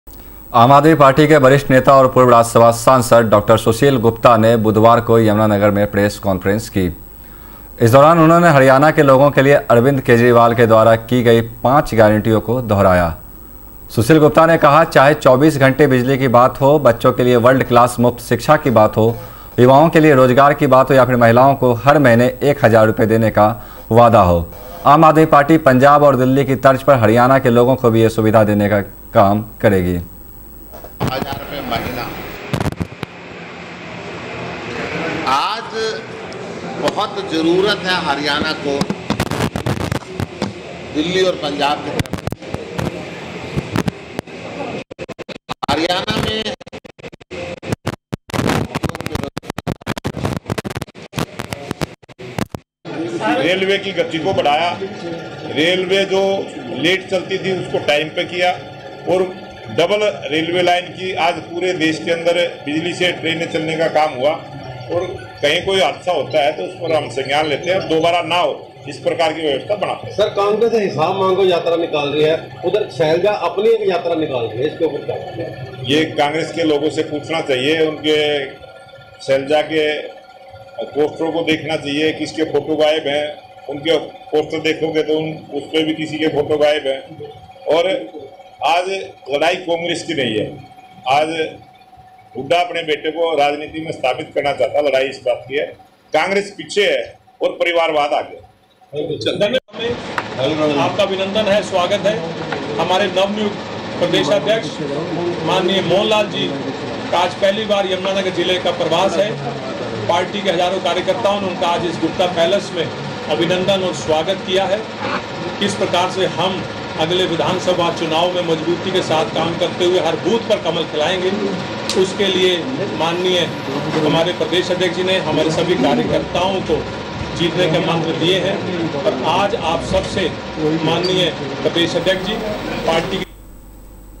यमुनानगर : यमुनानगर में पहुंचे आप नेता सुशील गुप्ता बीजेपी पर बरसे
Raftaar Media